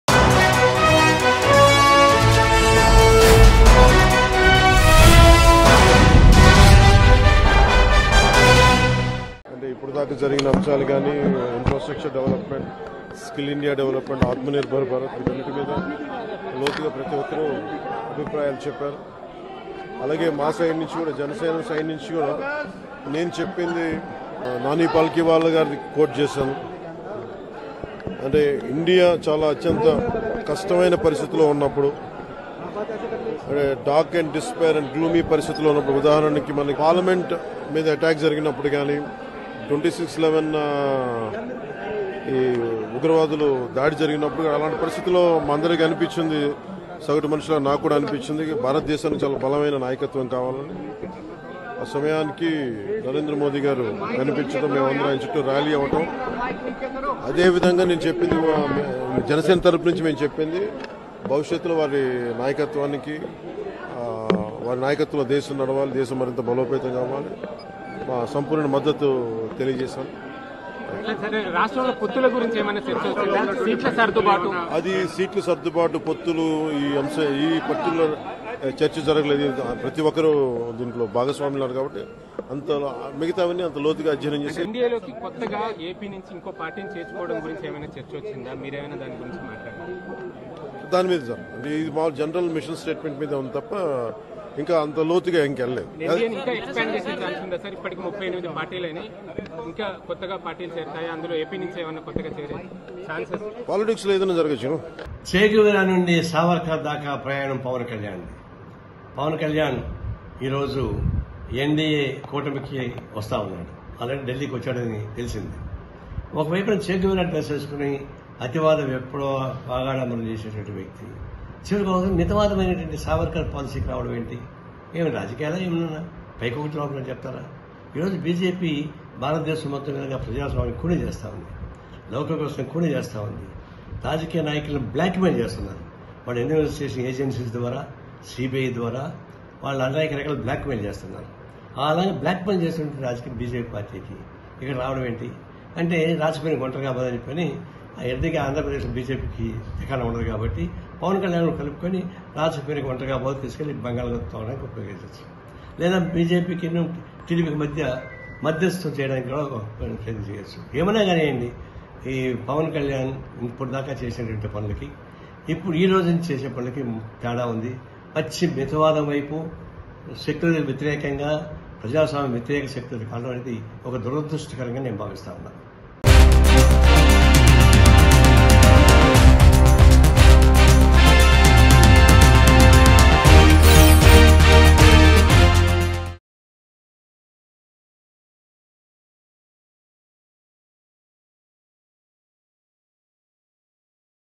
Combat Of Words Between Pawan Kalyan And CPI Narayana | Janasena Vs CPI | AP Politics | Mango News
Combat Of Words Between Pawan Kalyan And CPI Narayana.